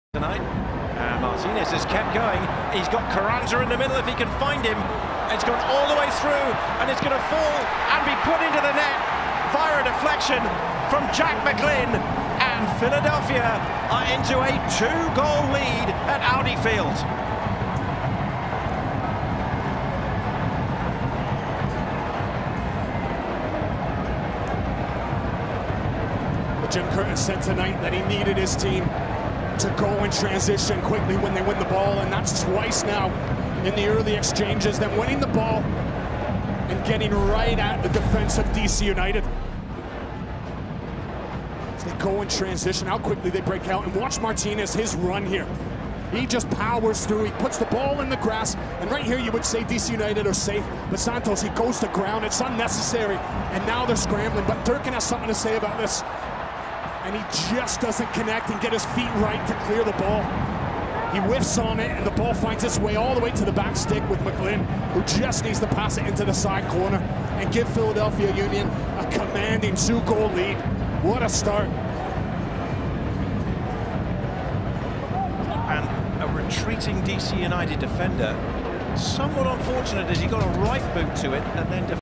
La cuarta asistencia de José “Brujo” Martínez en la MLS - DC United vs. Philadelphia Union
José Andrés Martínez asistió el segundo tanto en el triunfo del Philadelphia Union sobre DC United. Doce minutos después se retiró lesionado.

📽️: MLS.

📰 ¡Las noticias más recientes del mundo del deporte!